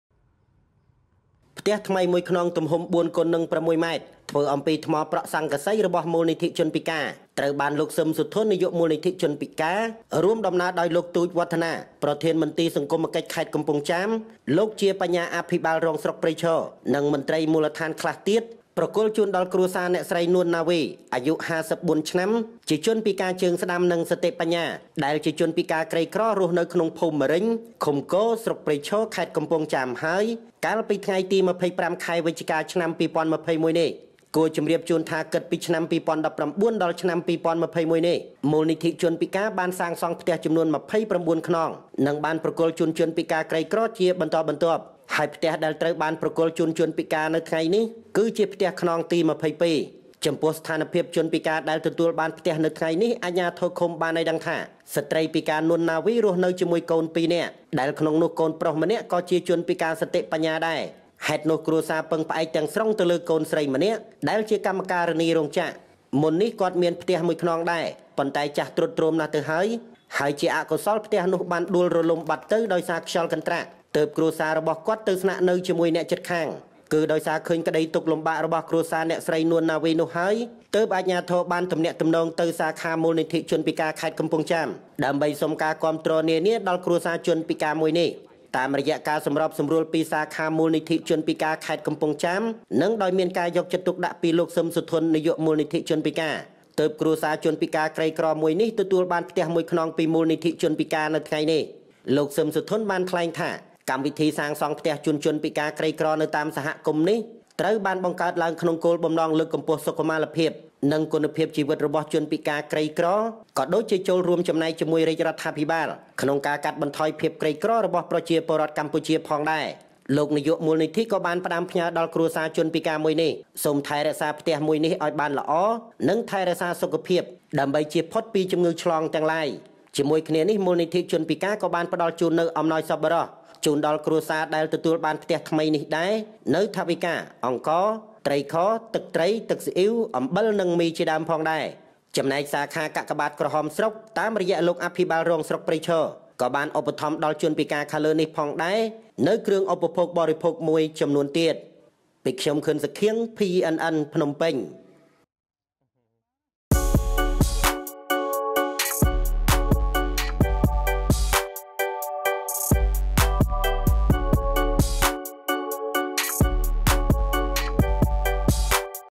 [26-11-21] ផ្ទះខ្នងទី២២ របស់មូលនិធិជនពិការ ត្រូវបានប្រគល់ជូនគ្រួសារជនពិការក្រីក្រ នៅខេត្តកំពង់ចាម
ផ្ទះខ្នងទី២២ របស់មូលនិធិជនពិការ ត្រូវបានប្រគល់ជូនគ្រួសារជនពិការក្រីក្រ នៅខេត្តកំពង់ចាម